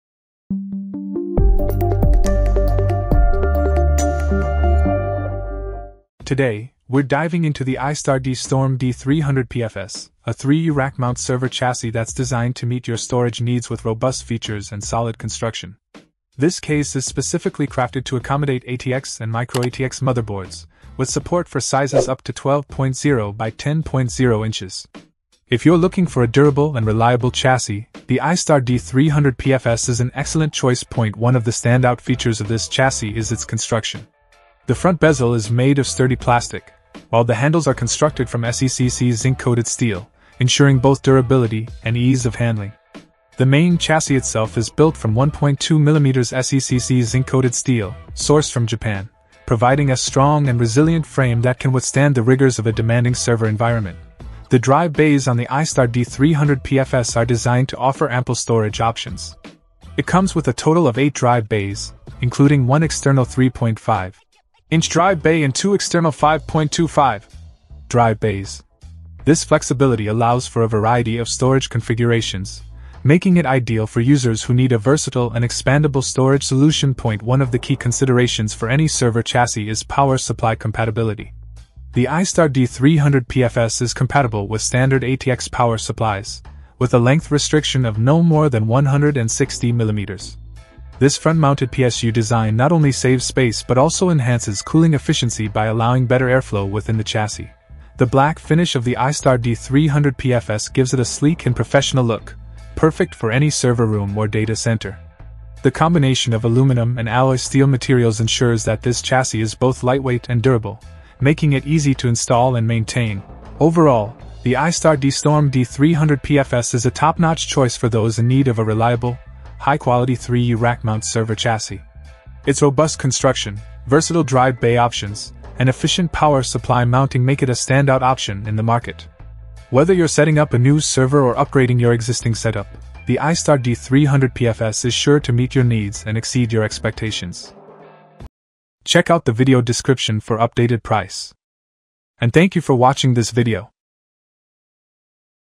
iStar D Storm D-300-PFS: The Best 3U Rackmount Server Chassis for Your Storage Needs
0:00 Intro
0:06 Review

Things that we mentioned in this video:
iStar D Storm D-300-PFS : B003U03GP8